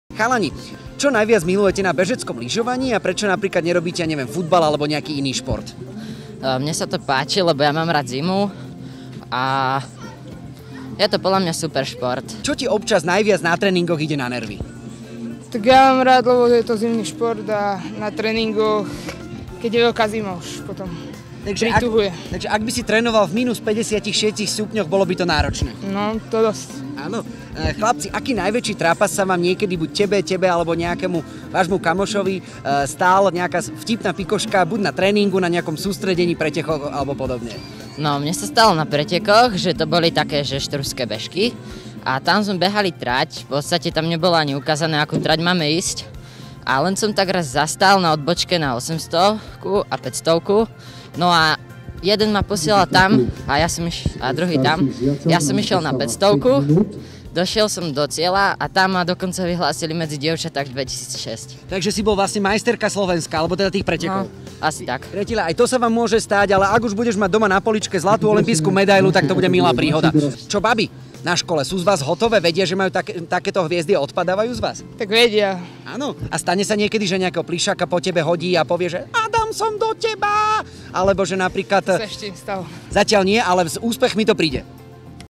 Sebastián Ilavský a Adam Černák
Aktuálni Majstri Slovenska pre rok 2018 v tímovom šprinte medzi staršími žiakmi reprezentujú klub ŠK Štrba.